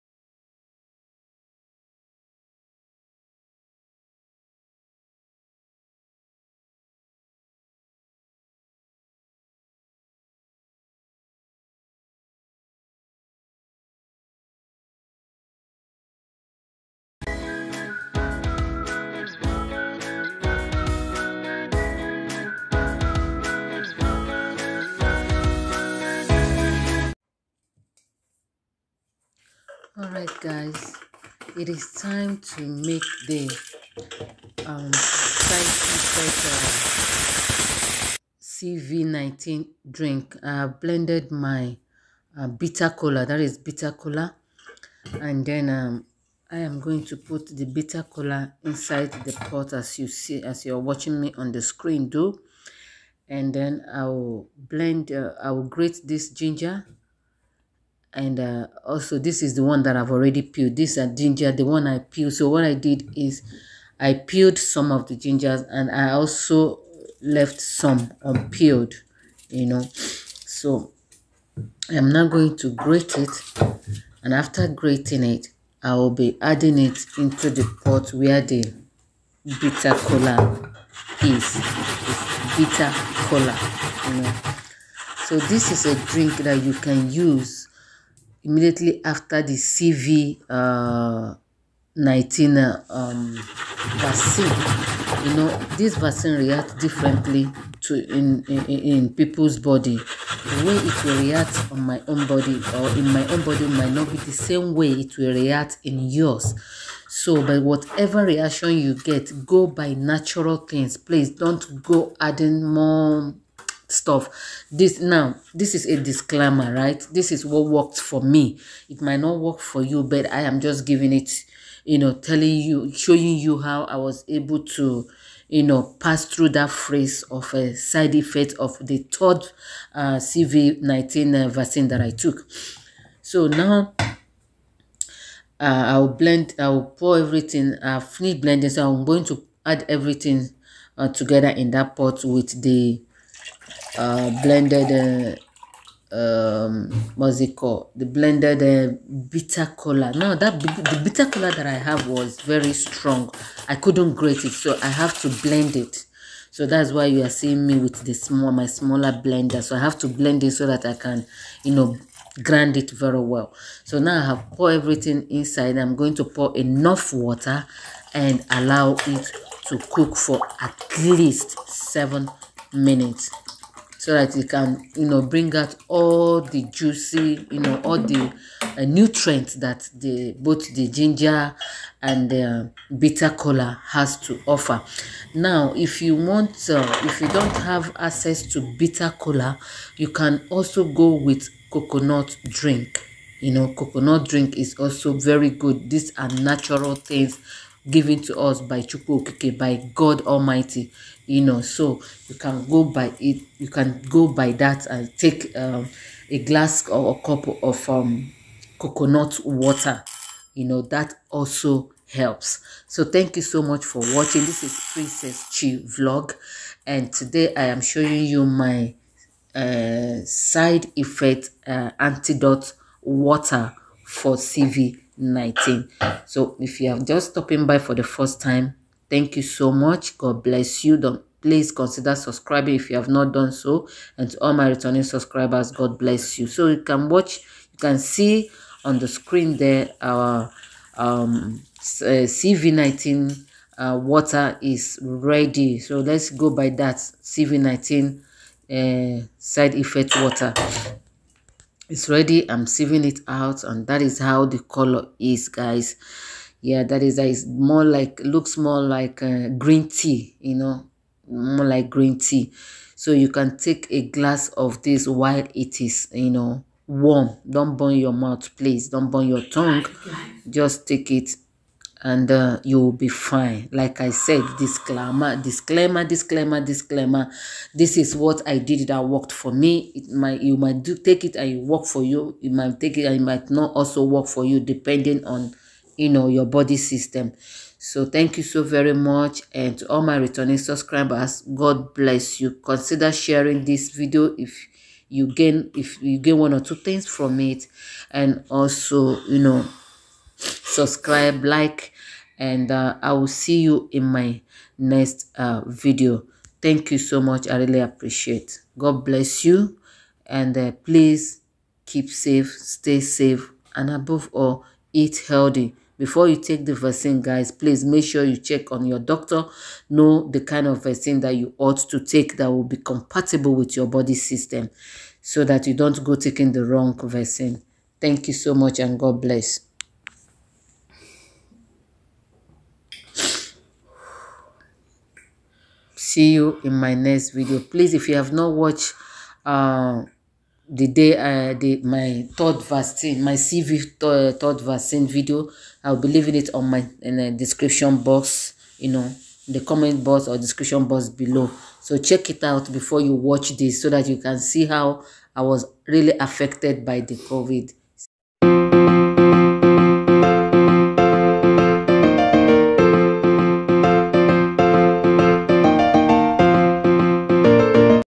DIY BITTER COLA and GINGER DRINK FOR CV19 VACCINE SIDE EFFECT.
In my previous videos, I showed you guys when I took my First,Second & the Third Vaccines.  Actually I didn't experience any side effects on my first and second vaccine,but the Third one was different. If you have not watched those videos, please consider clicking the links below to watch. So today I will be showing you guys the DIY drink I took that made me feel better.

Everything I wrote and showcased on my video is based on my own personal experience.  please Consult your doctor before taking anything. Thanks.